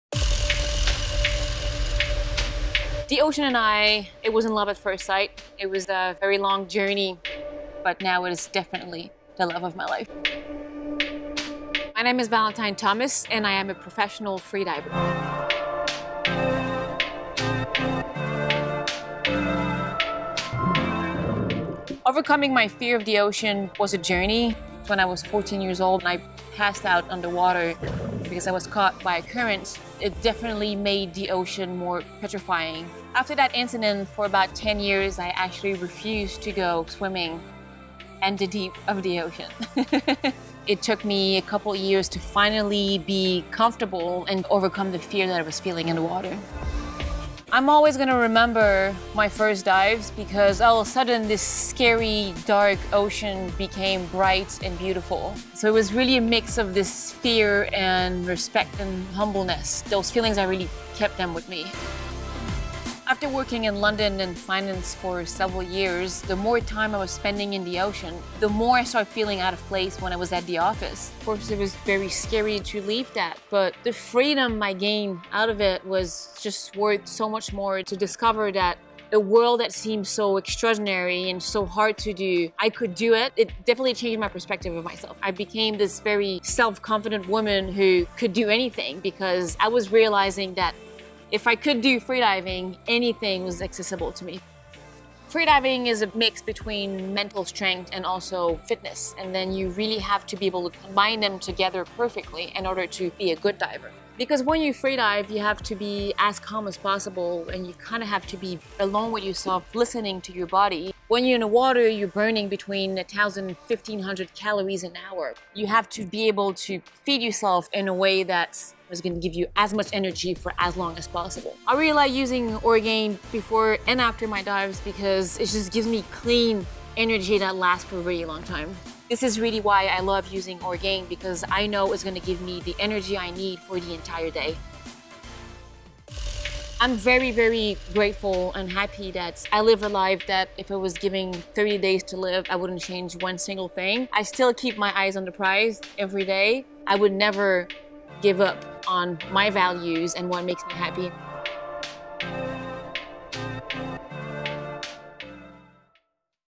Orgain Story with Professional Free Diver - Valentine Thomas
Professional Free Diver, Valentine Thomas, shares her story about her day to day life spent in the ocean and how she uses Orgain protein shakes for clean energy to fuel her through her day.